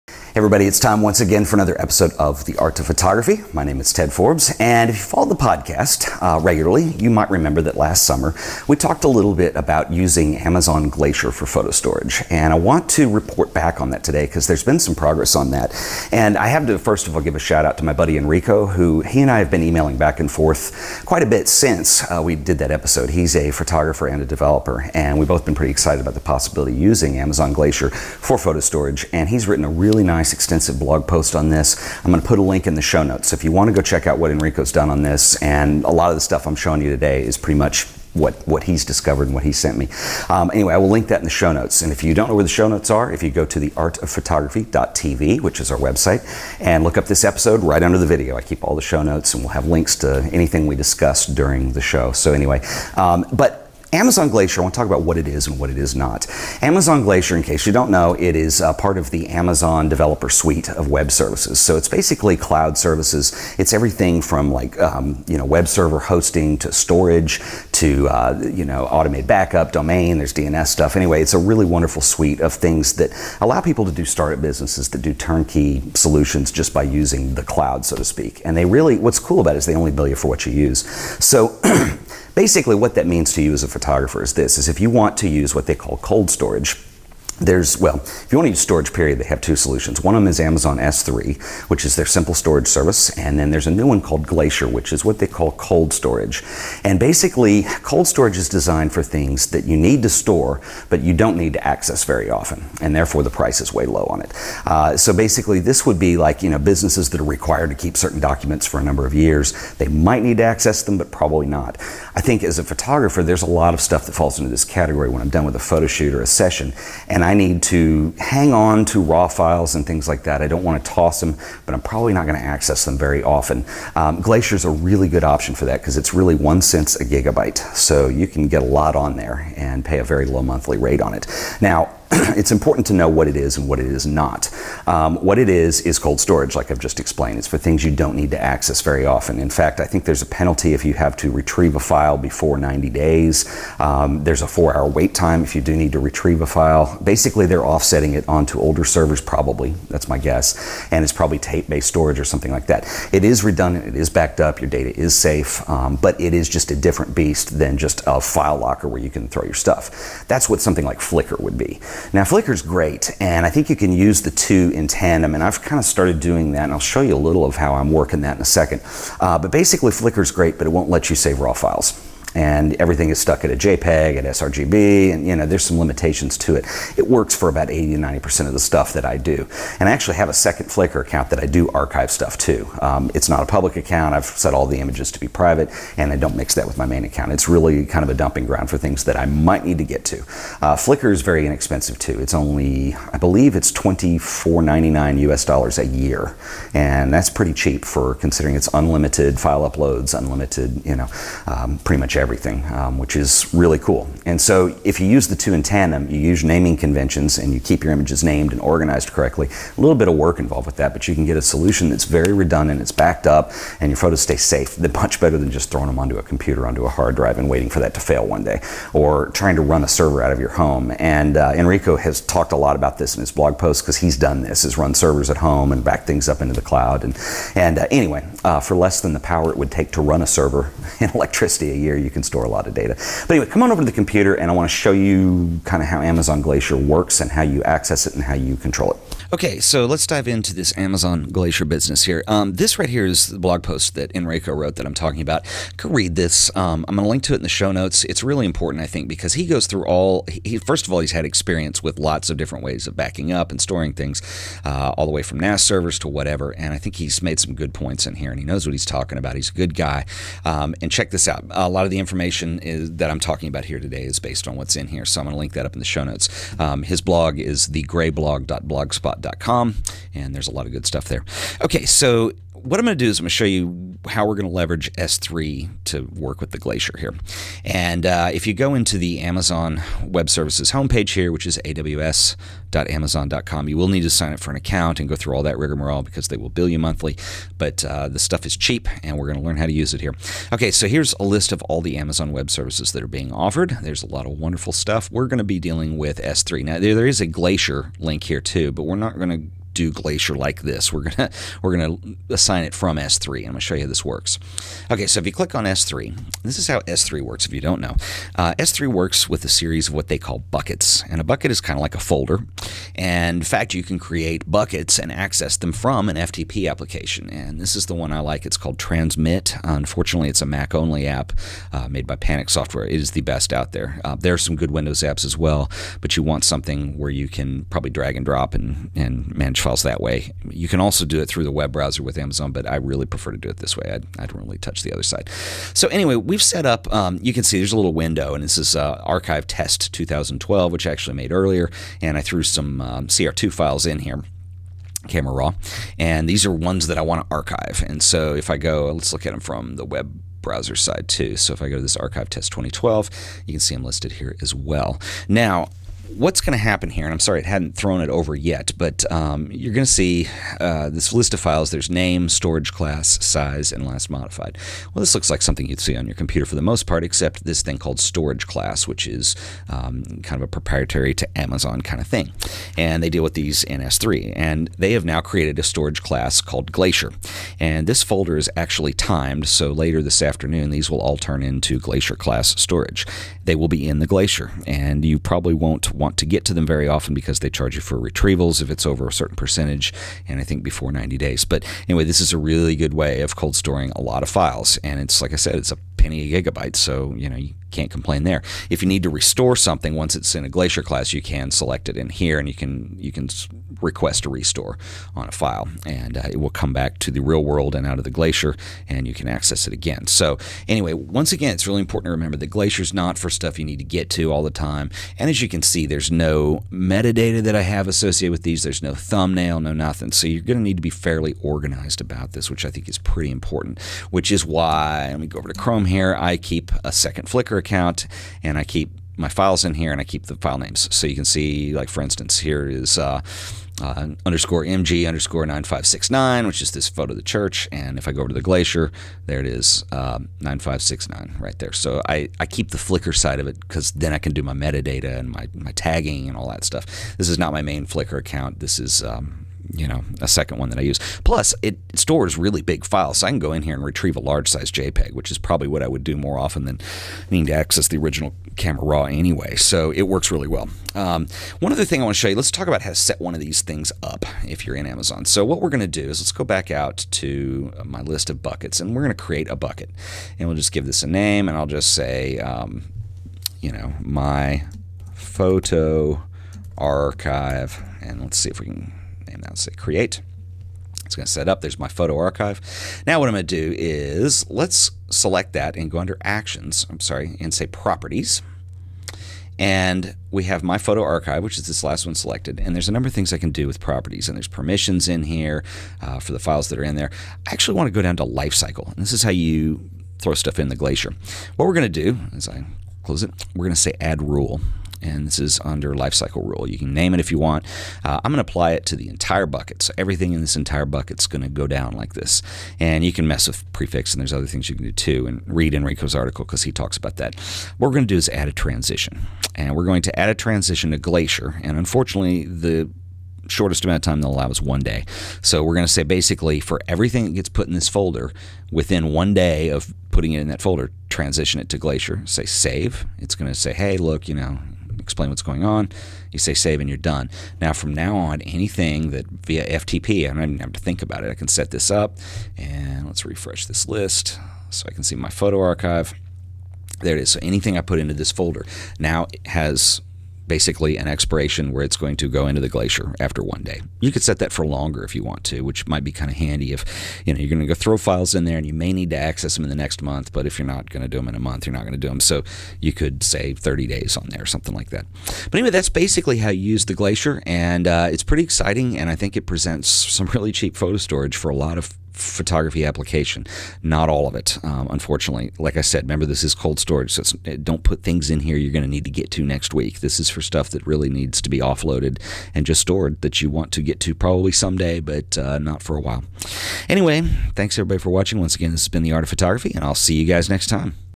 Photo Storage with Amazon Glacier and S3
All the info I'm covering is there as well as more detail.

Last summer we talked about the launch of Amazon Glacier and the possibilities of using it for photo storage. In this video I want to do a followup since its now actually possible to use Glacier and it turns out its a lot easier to use than we thought it would be when the announcement came.

Its extremely important here to say quickly what Glacier is and what Glacier is not.

Glacier is cold storage. This is designed for files that you don't need to access very often, but you don't want to delete. Cold storage is perfectly safe and redundant. Amazon doesn't explain the process, but most likely the files are offloaded on to tape based storage or servers that run low power at low temperature to reduce the cost of "running". This means if you need to get to one of these files, you'll need to initiate a request and 4-5 hours later your file will be ready for download. There is a transfer fee if you go over a percentage of your total storage.

Glacier is not an FTP server or Digital Asset Management System. You can't flip through thumbnails or in the use case I'm giving in this video, do meta data association.

But the great thing about Glacier is that you can store old photos that you don't need to get to very often. I like to use this in tandem with a second flickr account. I make sure my names for files are consistent and upload JPG files to Flickr and Raw files to Glacier. I use all the metadata and set management in Flickr and keep my files organized so that if I ever do need the raw file - I can retrieve it from Glacier.

The best part is if you can deal with these restrictions, Glacier is 1 cent per gigabyte per month. Way cheaper than it would cost to run a server in your house and its probably more practical as well.

In this example - we use S3 (which is also pretty affordable at 9 cents a gig) - but I'll show you how to change the storage class from Standard to Glacier. You'll just manage your Glacier files in S3 - no front end, fancy app or database management needed!